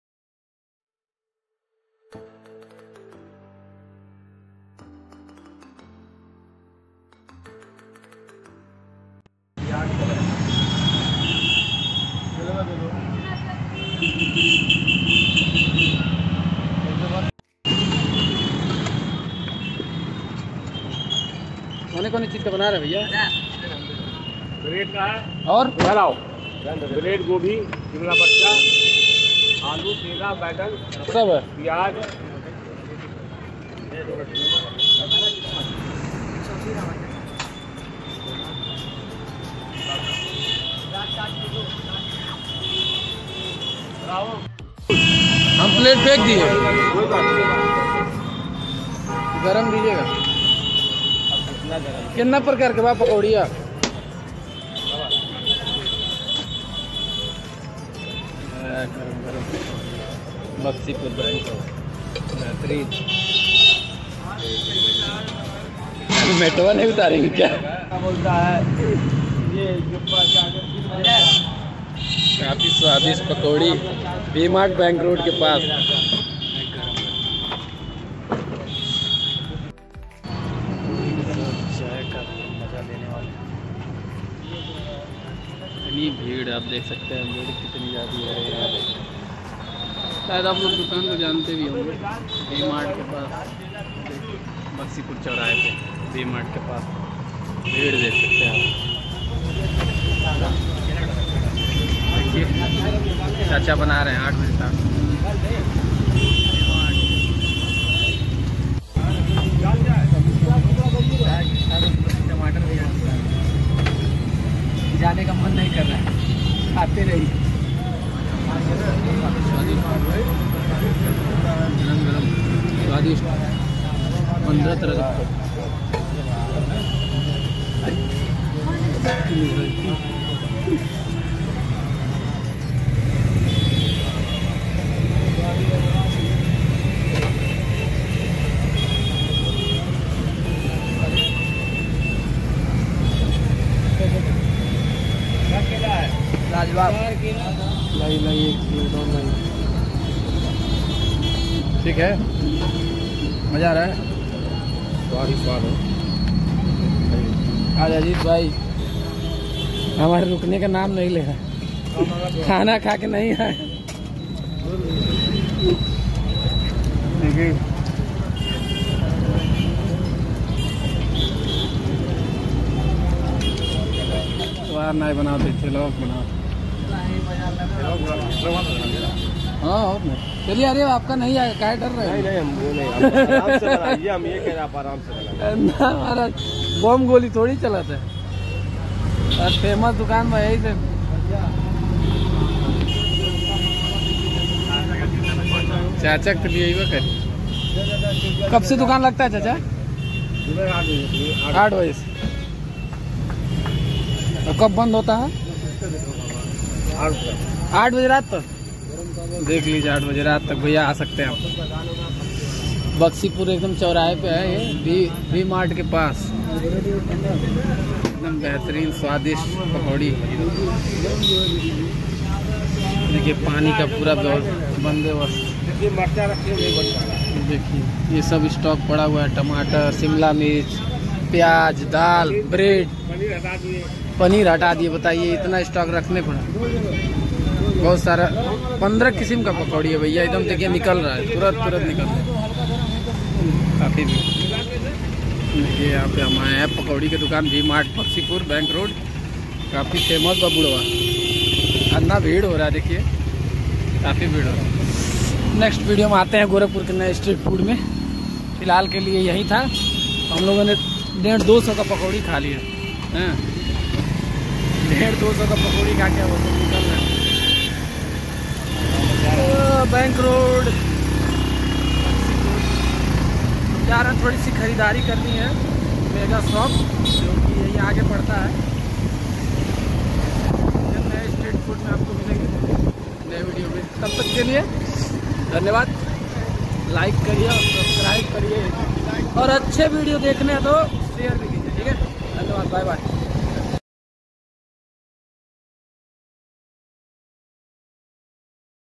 15 प्रकार की पकौड़ी।। स्वाद ऐसा की 150 की खा ली।। street food।Gorakhpur street food।।@Vikramfoodvlog
यह दुकान बक्सीपुर के बैंक रोड पे उपलब्ध है।।।यह काफी कम दाम में 15 प्रकार की पकौड़ी मिलती है।।यह वी मार्ट के पास विपरीत दिशा में दुकान लगती है।। प्राइस मात्र 3,5,10 रुपए में।।